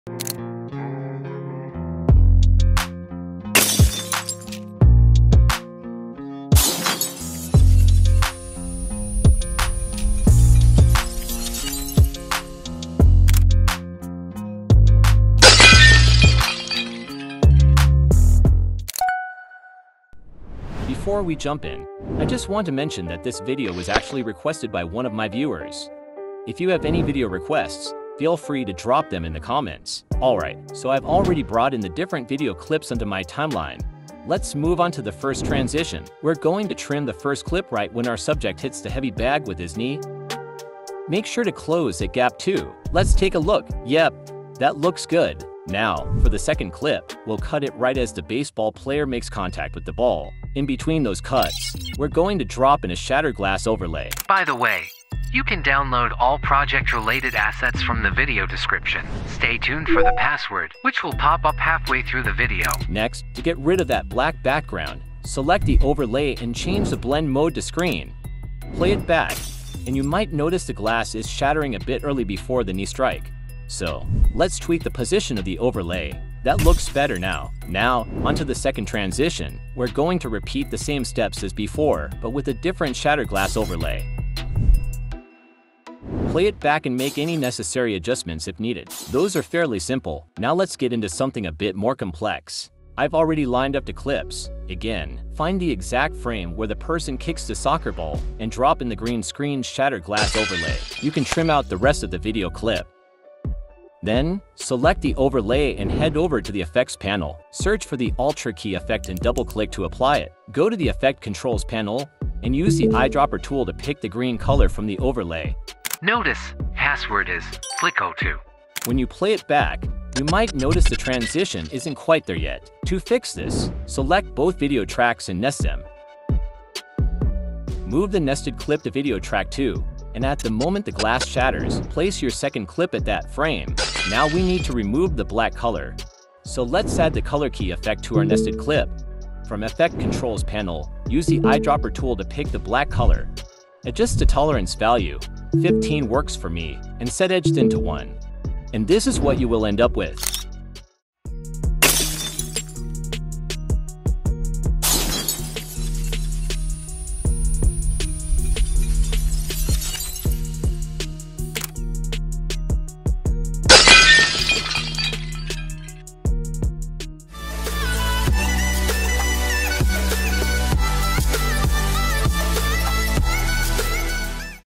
3 SHATTER GLASS Transitions in Premiere Pro
In today's video, we're diving into the world of video editing with "3 SHATTER GLASS Transitions in Premiere Pro!" If you're looking to add a dynamic and eye-catching flair to your projects, these transitions are perfect for you!

🕝Timestamps:
00:00  Intro
00:21  Set Up
00:34  Shatter Glass Transition 1
01:17  Shatter Glass Transition 2
01:29  Shatter Glass Transition 3
02:30  Final Result